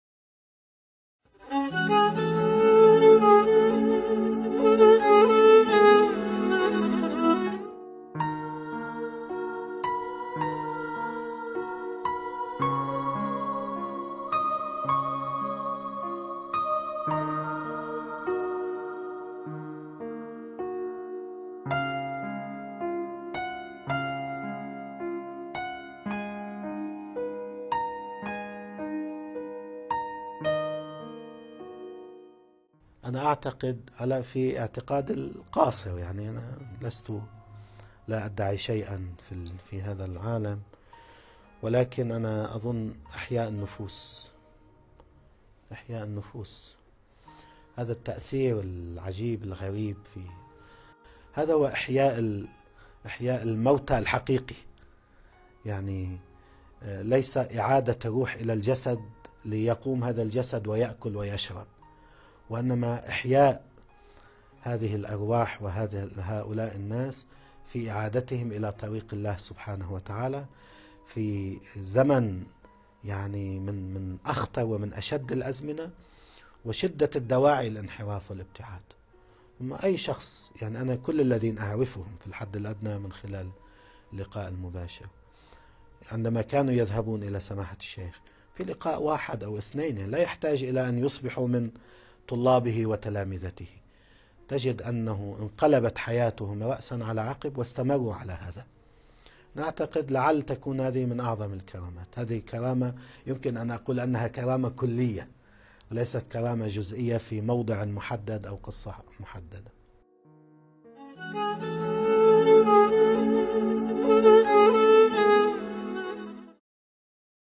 Di Antara Keramat Teragung Syekh Behjat adalah 'Menghidupkan Jiwa' Manusia
SAFINAH TV - Apakah keramat teragung Syekh Behjat di mata SHN?
Selamat Menyaksikan...